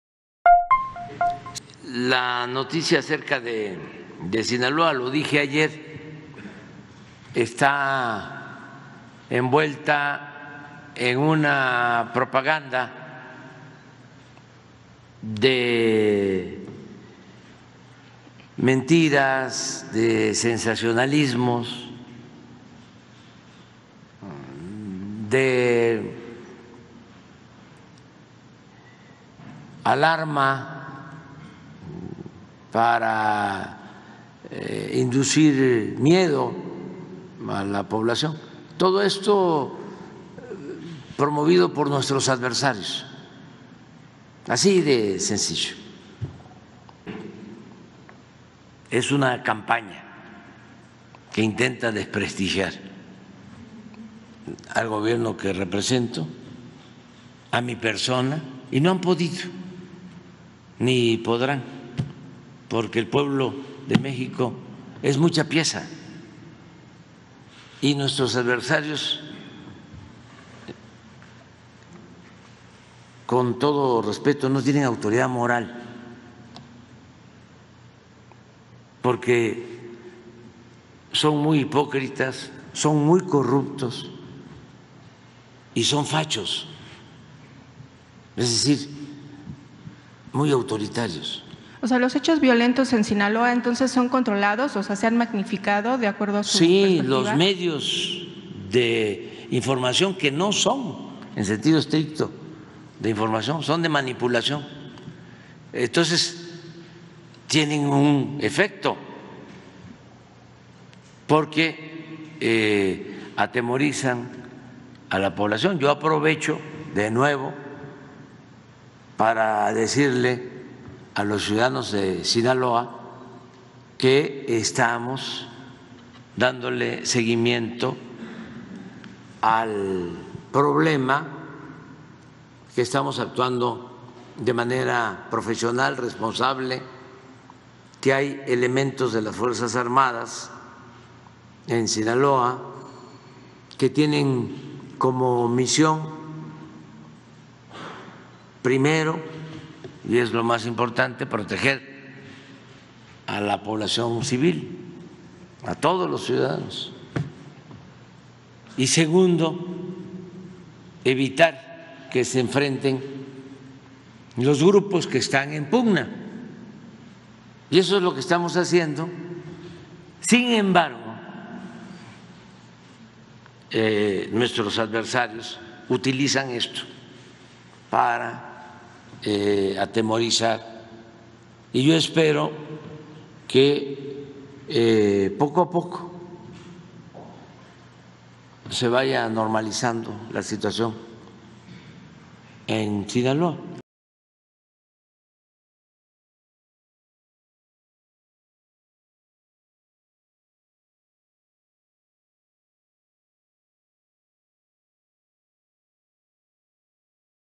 Medios magnifican violencia para atemorizar en Sinaloa.-AMLO
amlo conferenciamatutina presidente AMLO acusó a medios de comunicación de magnificar violencia en Sinaloa.